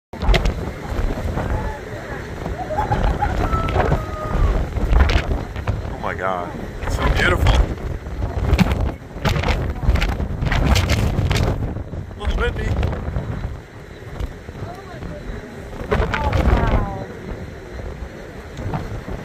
aurora
As viewed on board Nieuw Statendam on August 18,2022 just south of the Southern tip of Greenland.